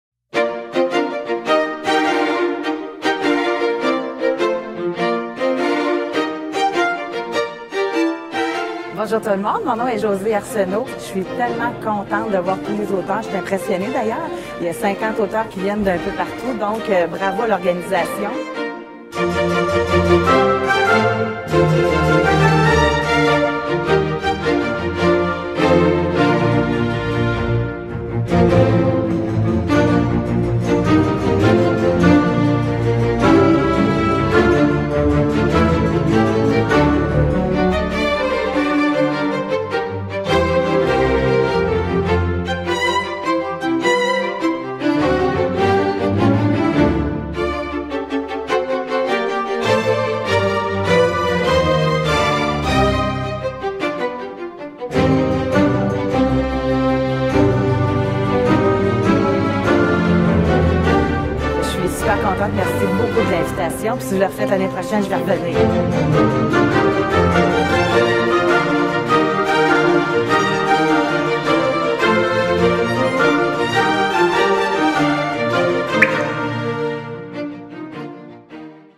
Salon du livre de Cap-Saint-Ignace 2018
Voici la capsule souvenir de la première édition du Salon du livre de Cap-Saint-Ignace 2018.